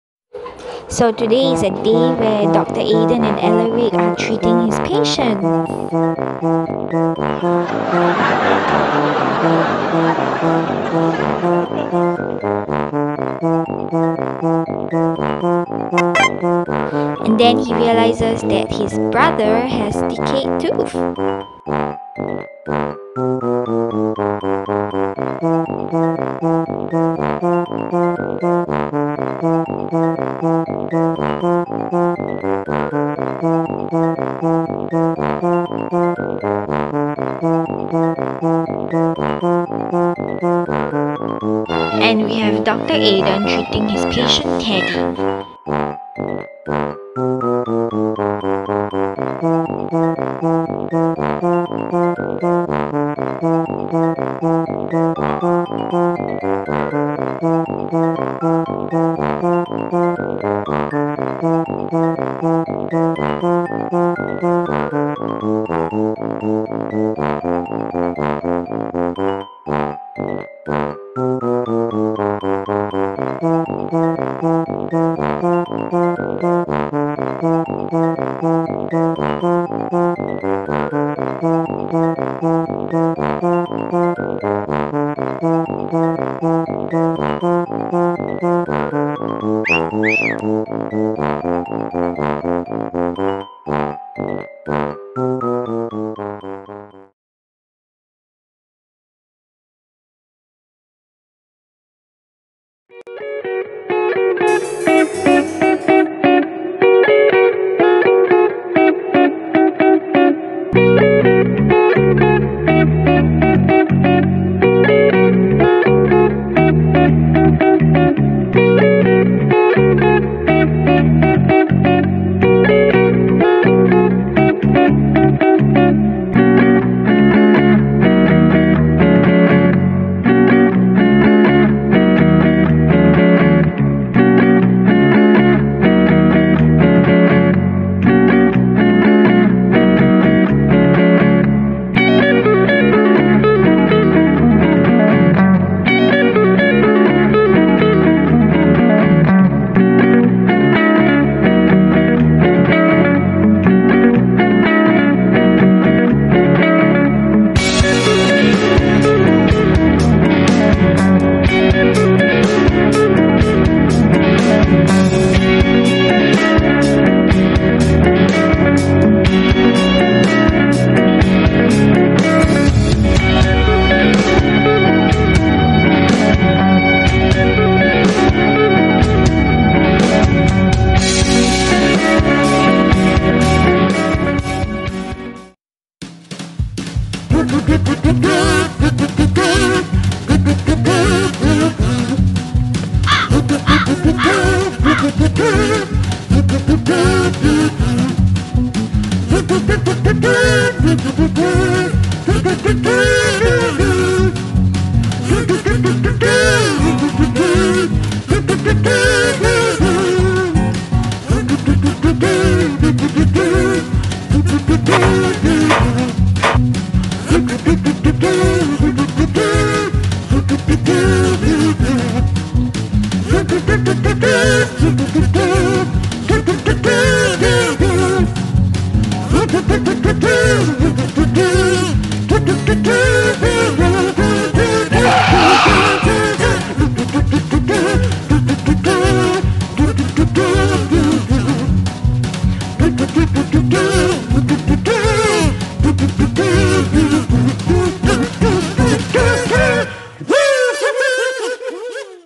THE DAY WE PLAY DOCTORS
Watch how we treat our patients teddy and ellie with such great care!